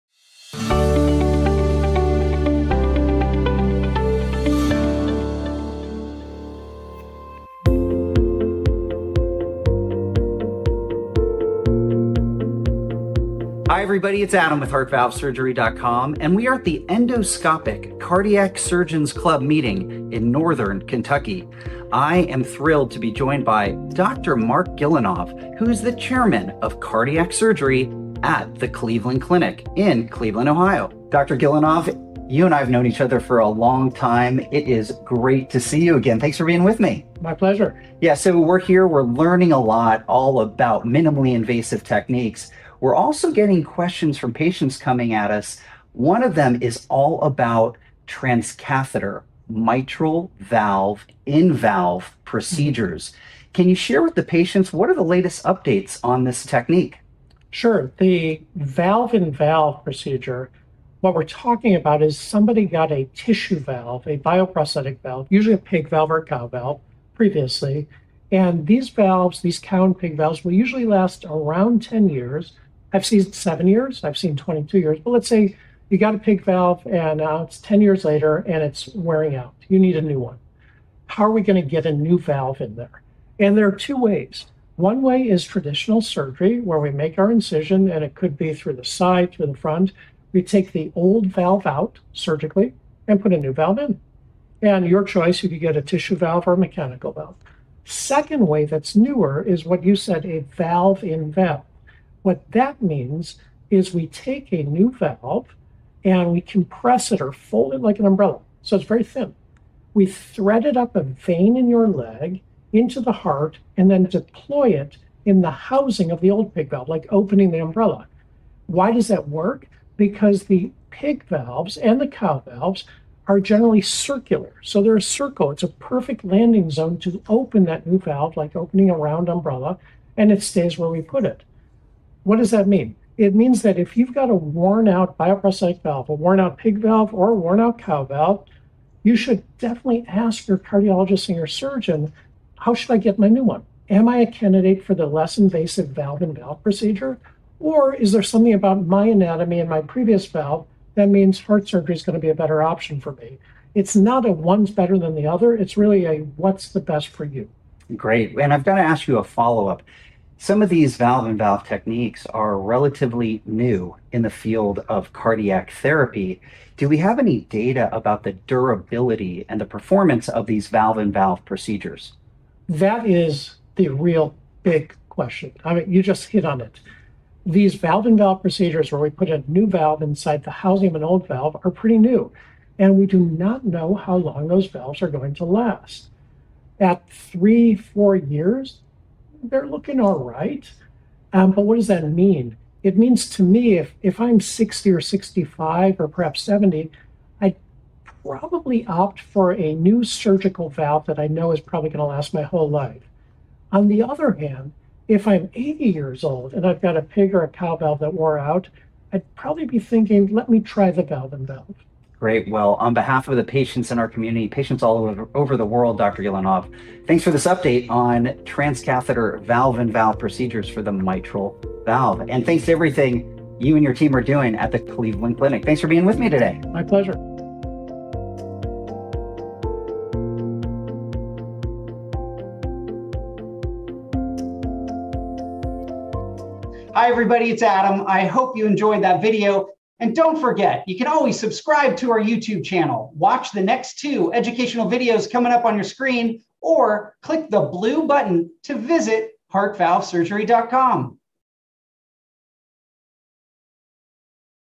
Surgeon Q&A: Transcatheter Mitral Valve-in-Valve Procedures
– If you have a failing tissue mitral valve replacement, do you always need another open-heart operation? Or could a less invasive transcatheter “valve-in-valve” procedure provide a new valve without traditional surgery?

To answer this important question, HeartValveSurgery.com founder Adam Pick spoke with Dr. Marc Gillinov, Chairman of Cardiac Surgery at the Cleveland Clinic in Cleveland, Ohio. In this educational video, Dr. Gillinov explains why tissue (bioprosthetic) mitral valves wear out over time, how transcatheter valve-in-valve techniques work, and which patients may be candidates for this newer approach.

In this video, Dr. Gillinov discusses:
- How long tissue mitral valves typically last and why they eventually fail
- The two main options for a failing tissue valve: redo surgery vs. valve-in-valve
- How a transcatheter valve-in-valve is delivered through a leg vein and expanded inside the old valve
- Why some patients are better suited for surgery and others for a less invasive procedure
- What early data show about valve-in-valve durability and how age and health influence the choice